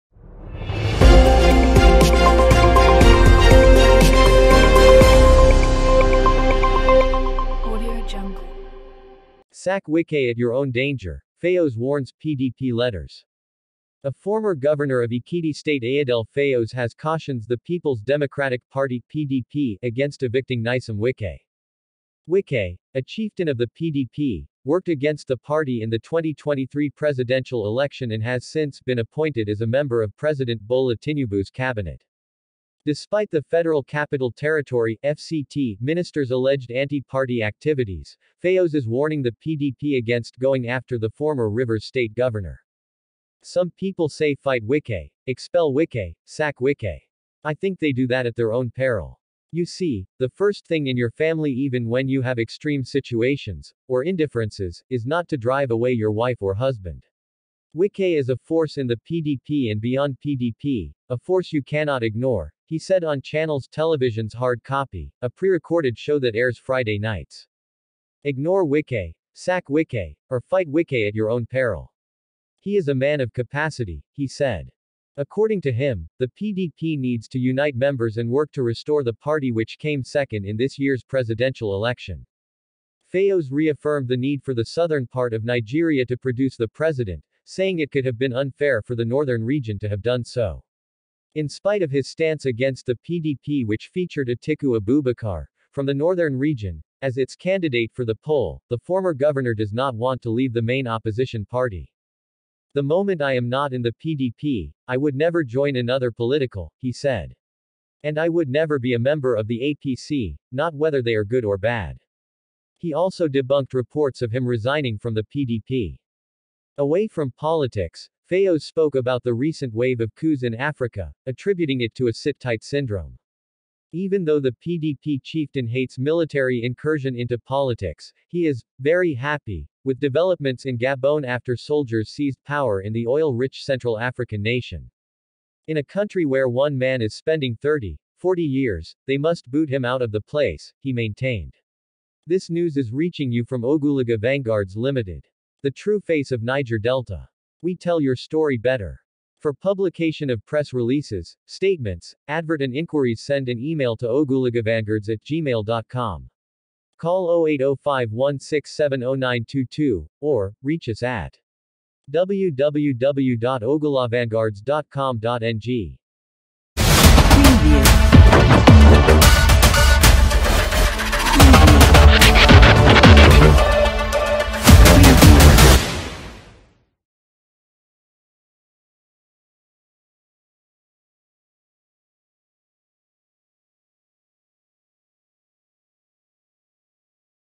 Sack Wike At Your Own Danger, Fayose Warns Pdp Leaders
OgulaghaVanguardsNews